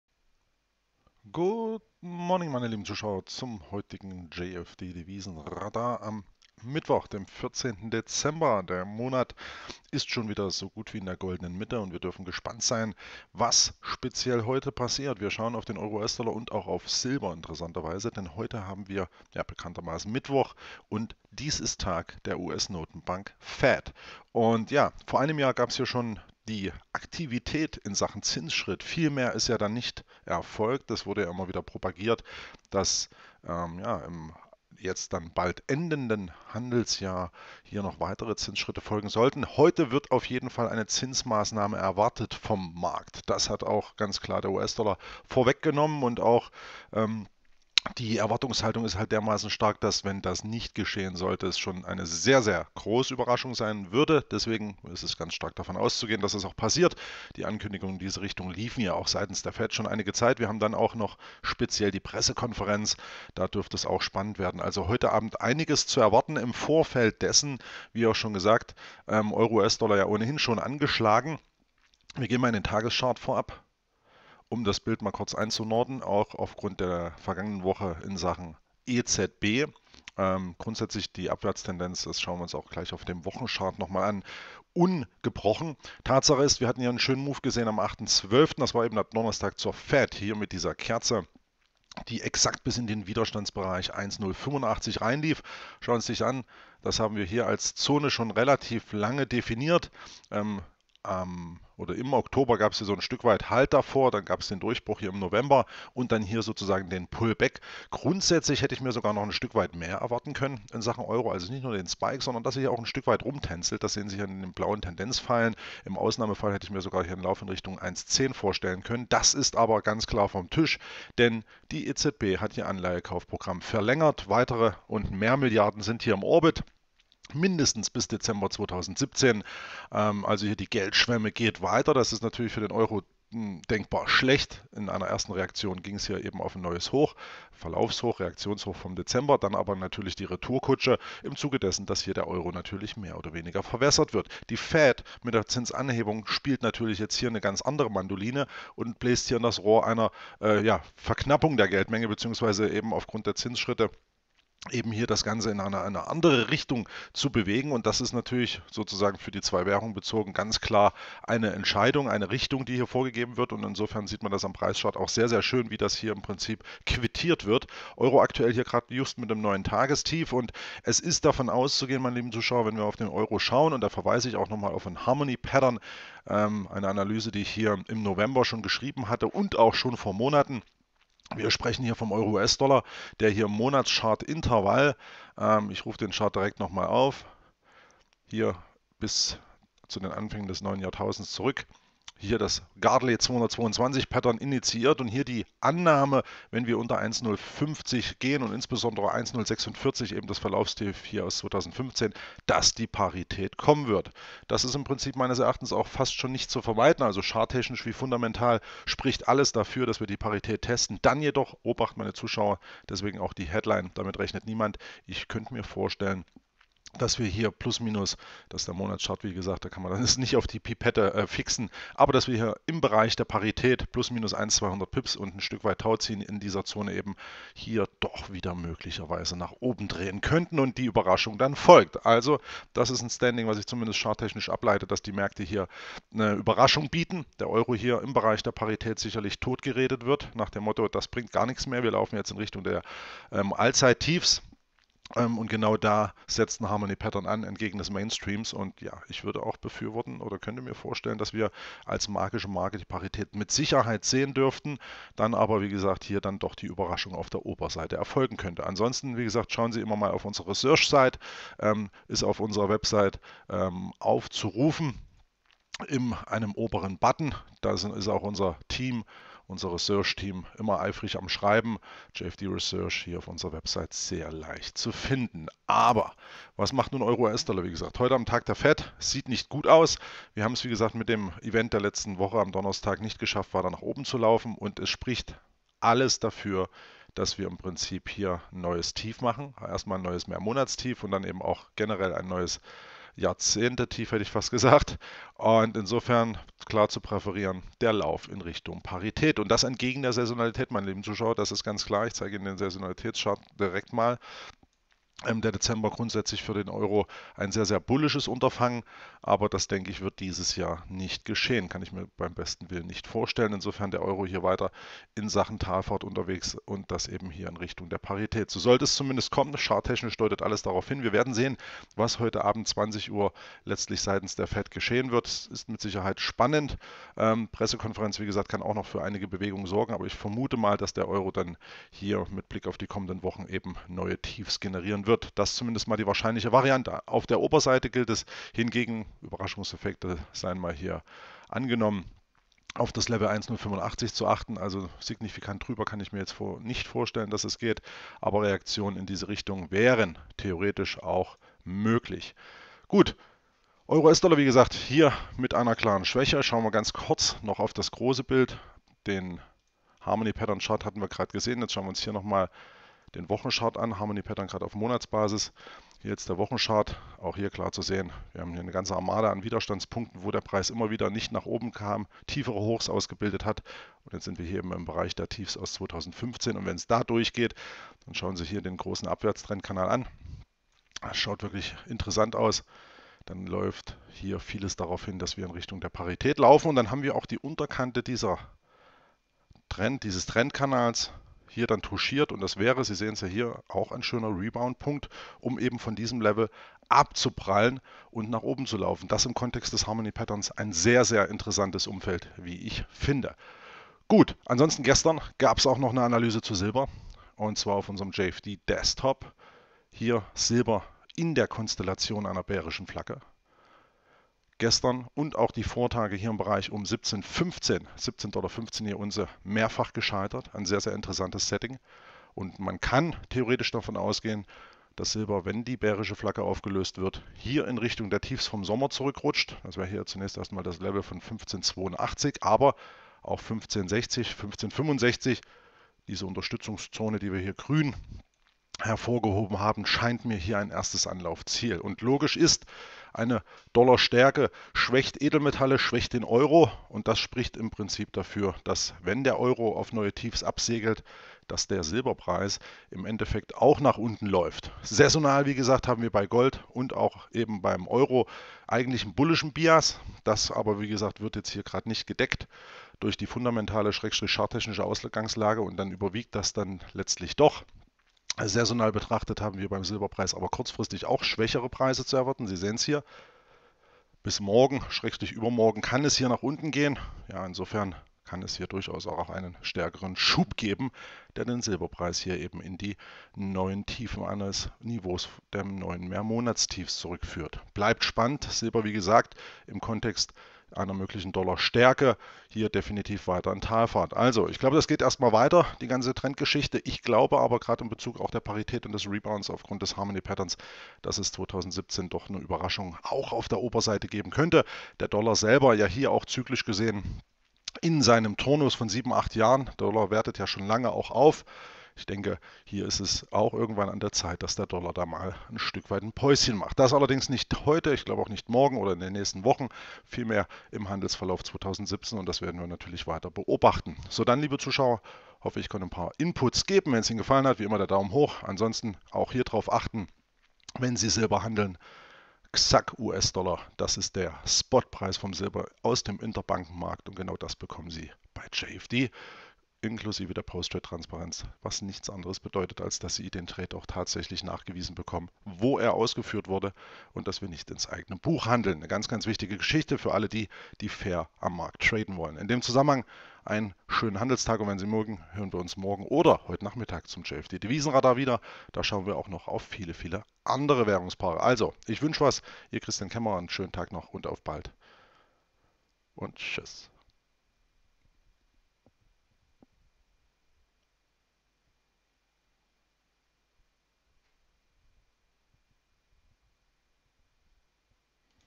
JFD Rohstoff- & Devisenradar: EUR/USD und Silber am Tag der FED
Die heutige Erwartungshaltung ist relativ klar. Die FED wird einen Zinsschritt tätigen. Diese fundamentale Tatsache begünstigt den Dollar schon seit einiger Zeit und schwächt die Edelmetalle wie den Euro.